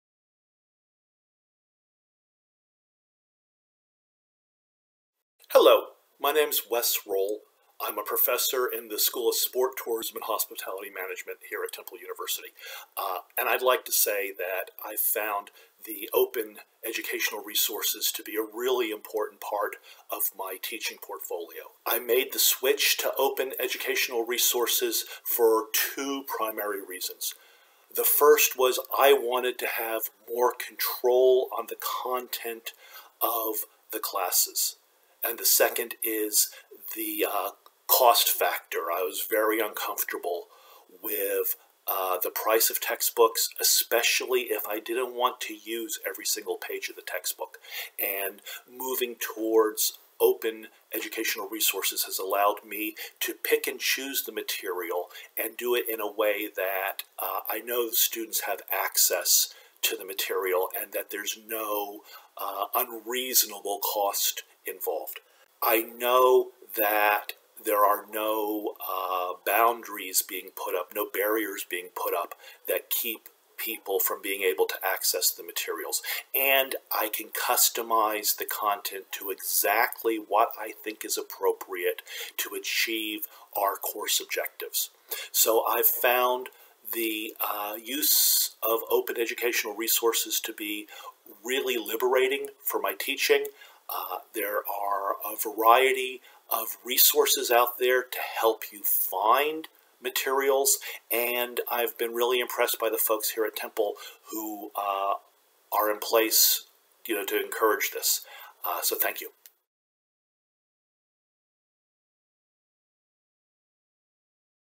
Wesley Roehl on OER
Temple University professor discusses switch to open educational resources